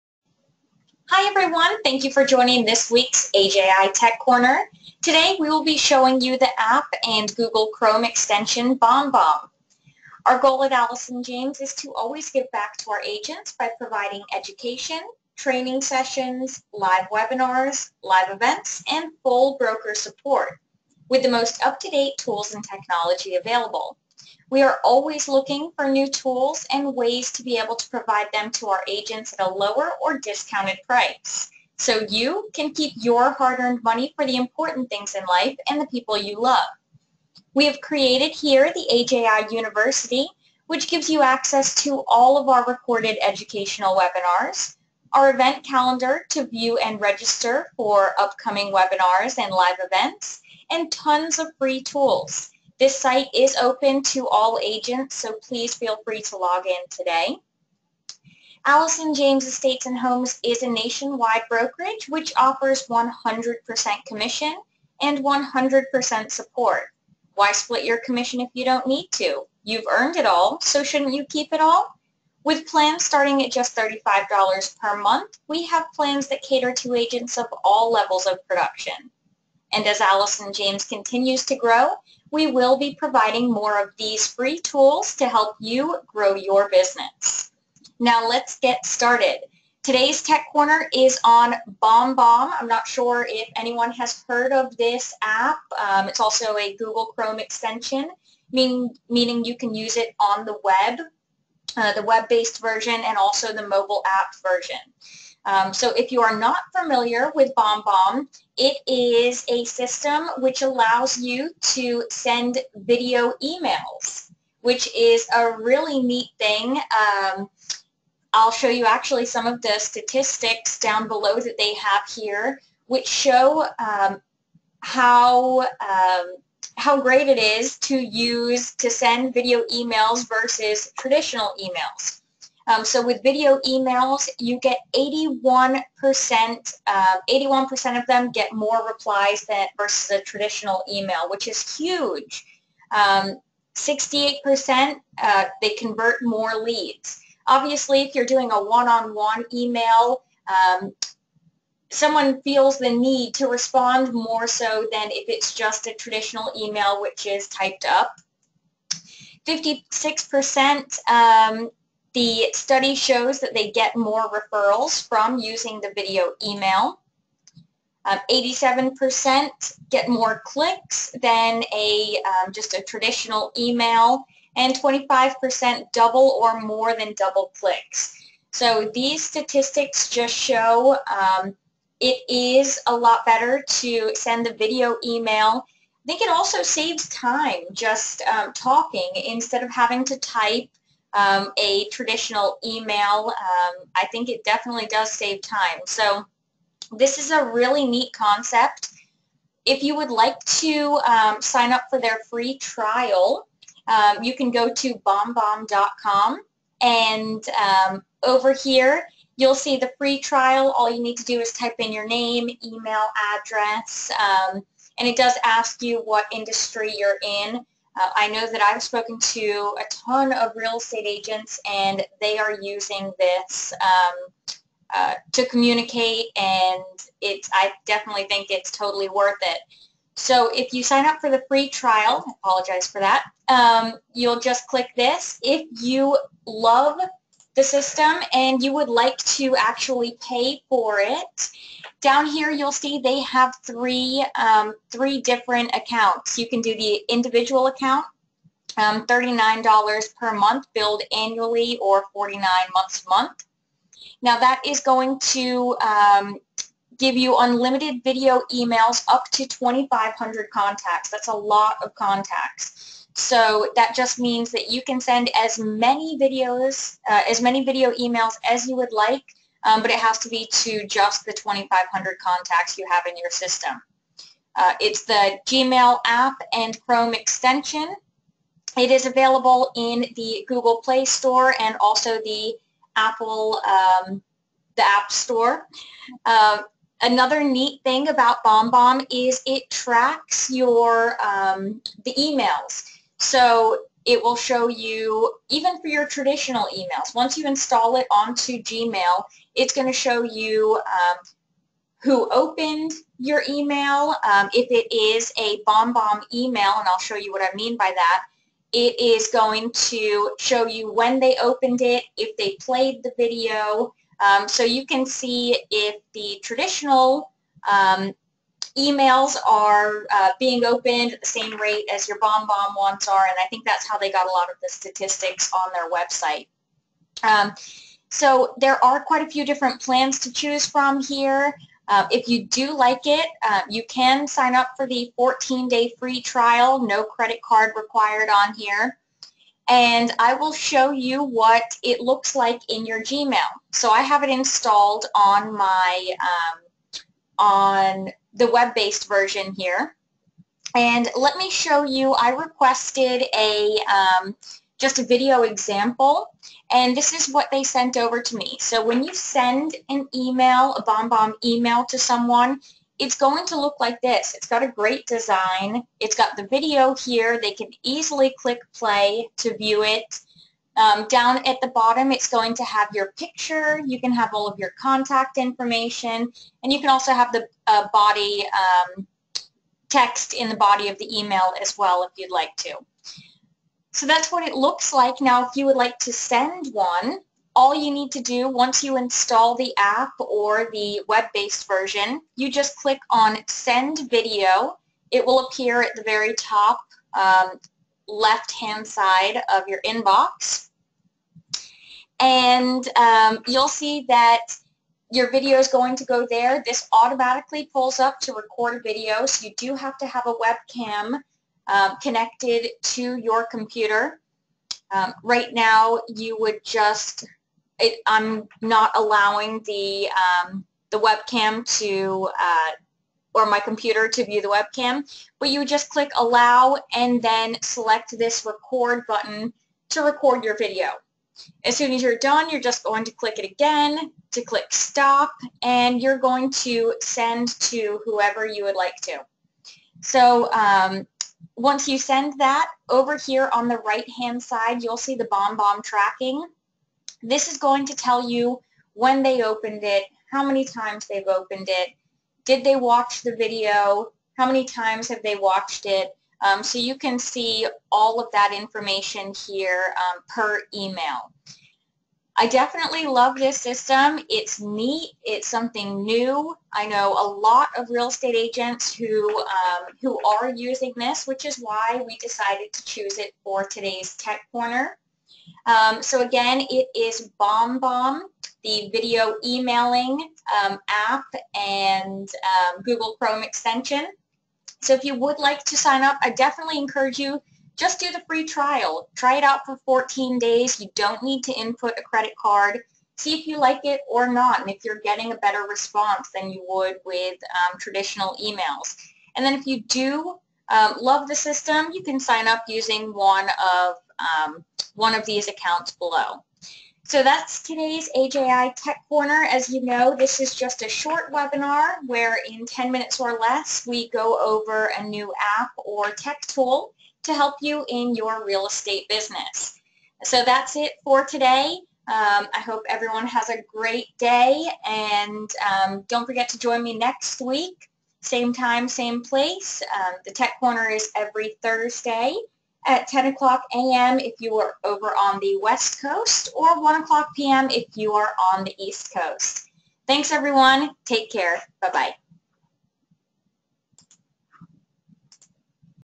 Bomb Bomb for Realtors®
In this week's AJI Tech Corner we go over the Google Chrome Extension Bomb Bomb where you can send video emails to your contacts! The statistics show that video emails convert more leads and get more replies than traditional emails. This is a great system to help boost your Real Estate Business!